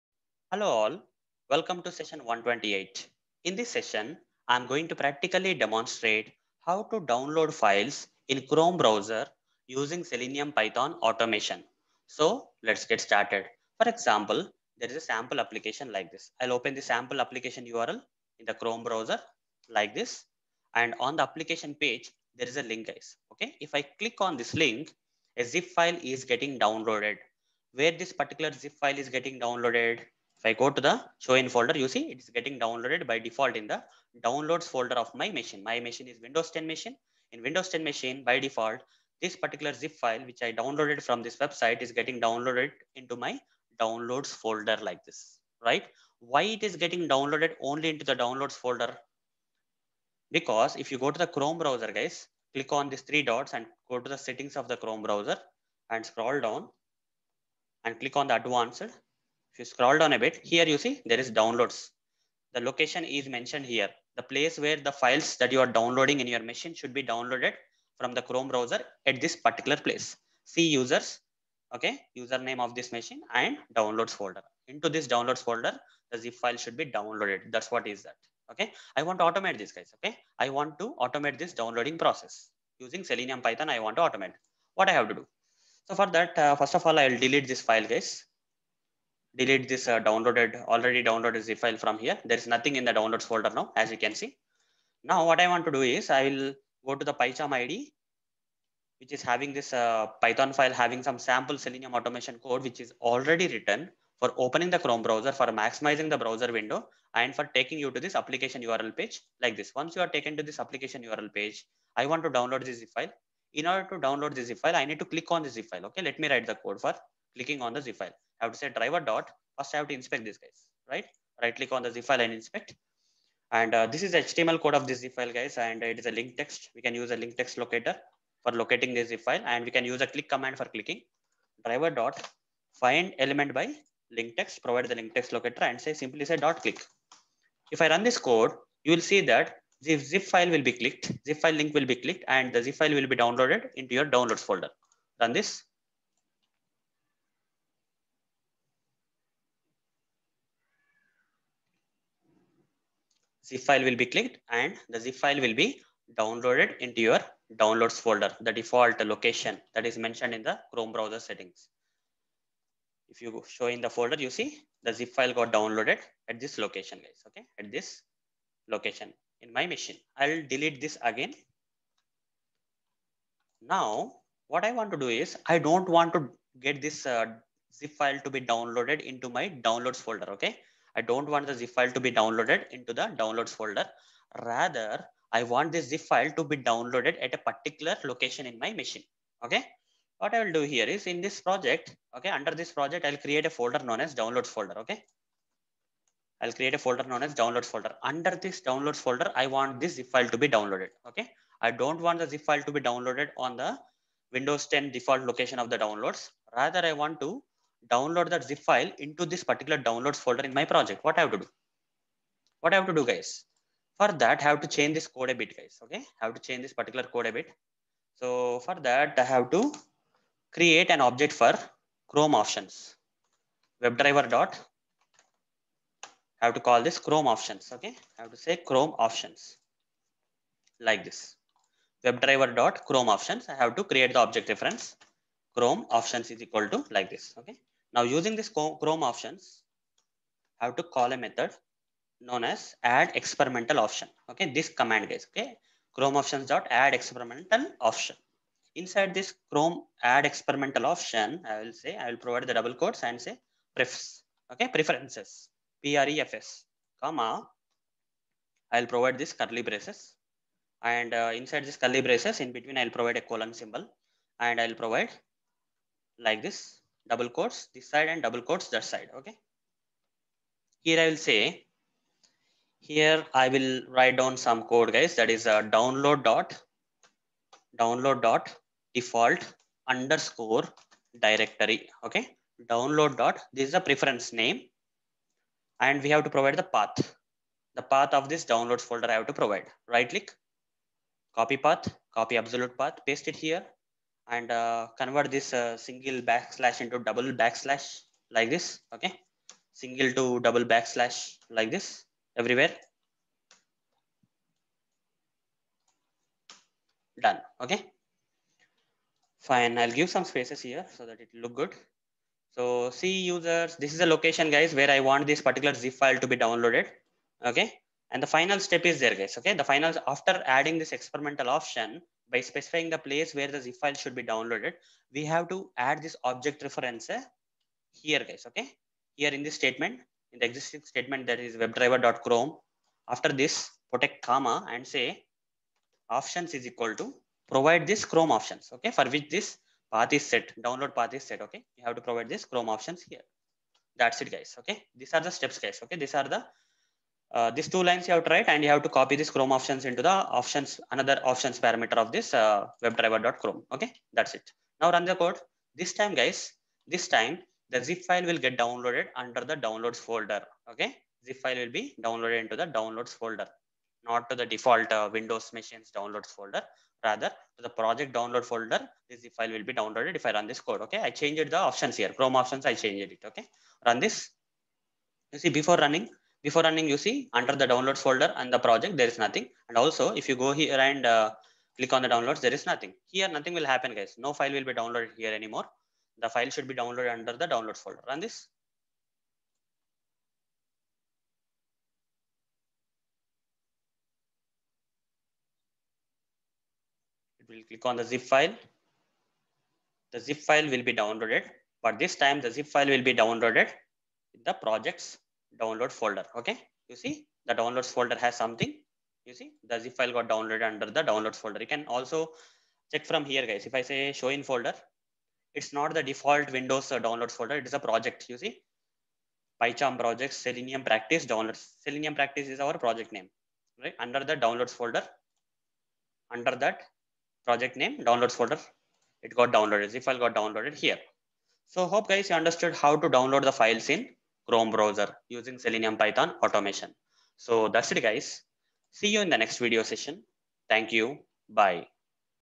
Downloading Files in Chrome Browser (Selenium Python - Session 128)
In this session, I have practically demonstrated downloading files in Chrome browser using Selenium Python automation.